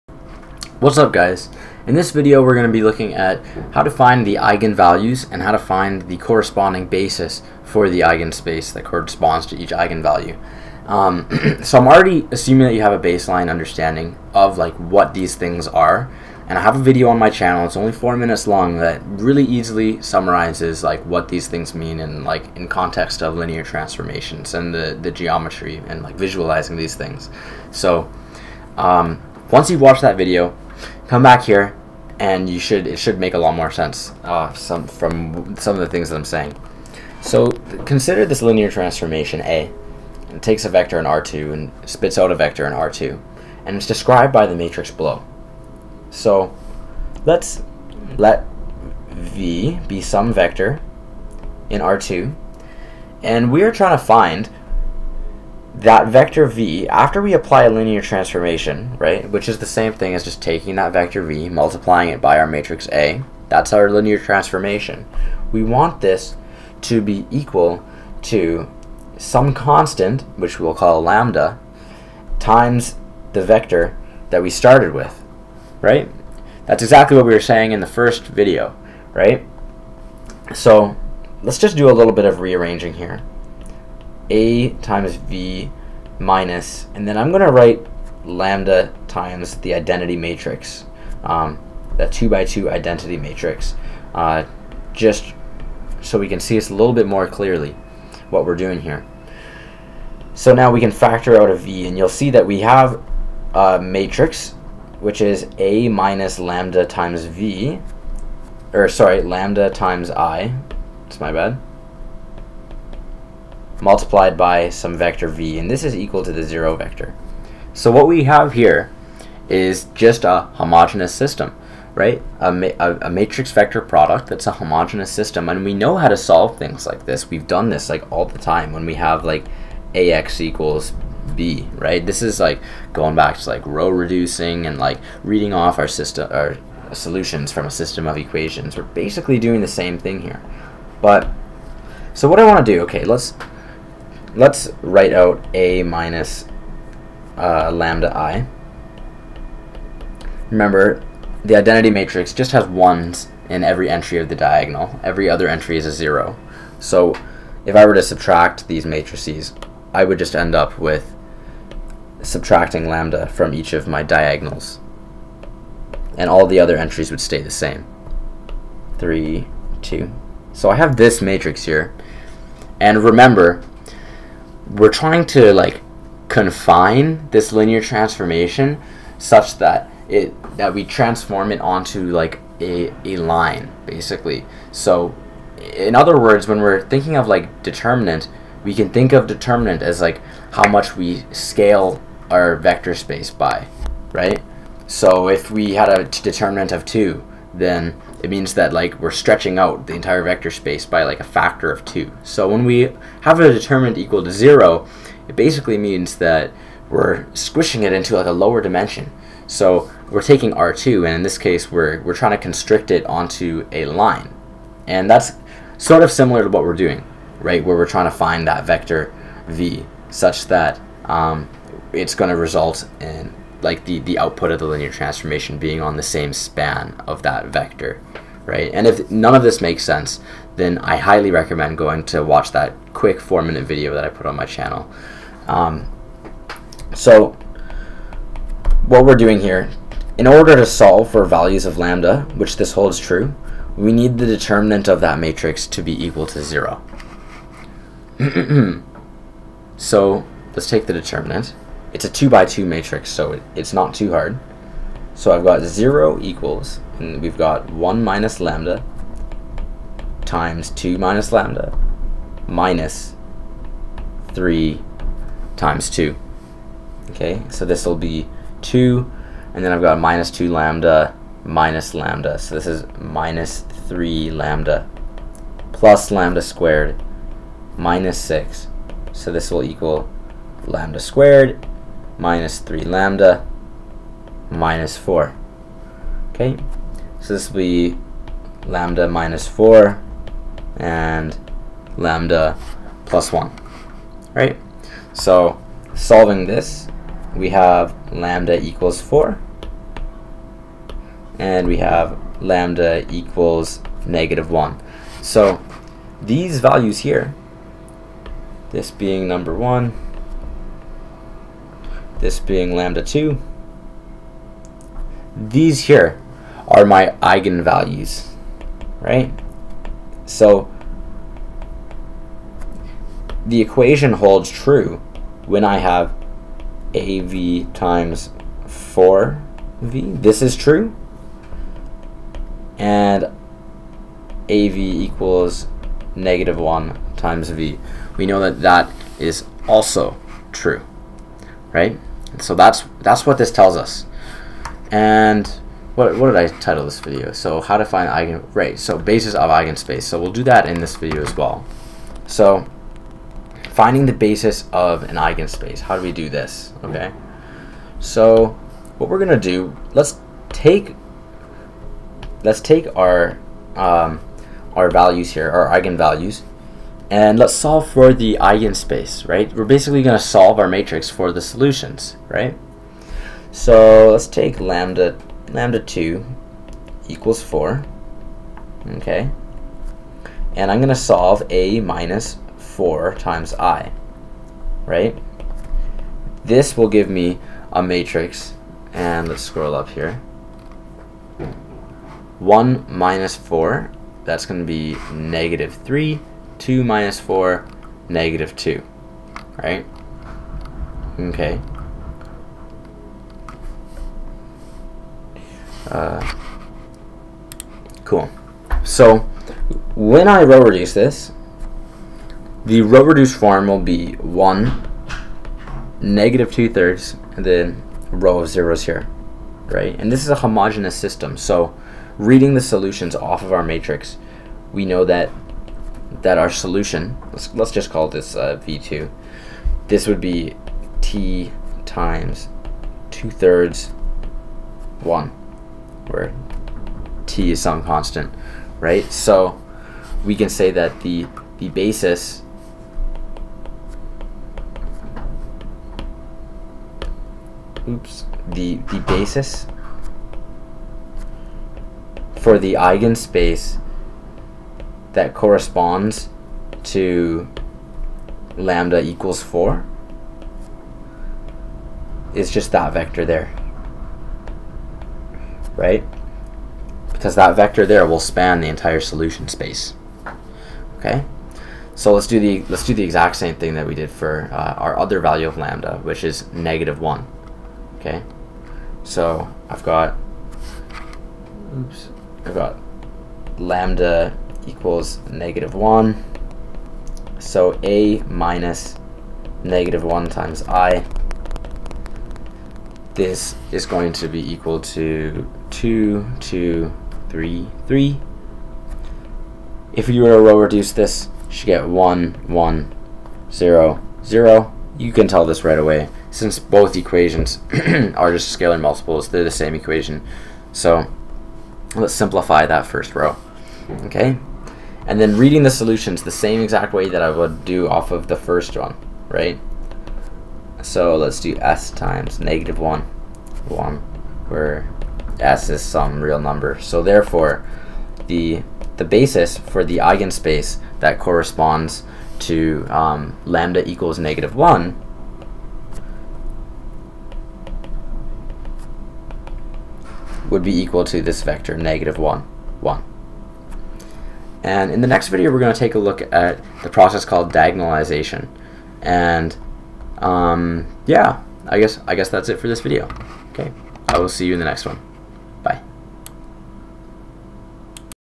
How to Find Eigenvalue and Basis for Eigenspace
In this video, we take a look at the computation of eigenvalues and how to find the basis for the corresponding eigenspace.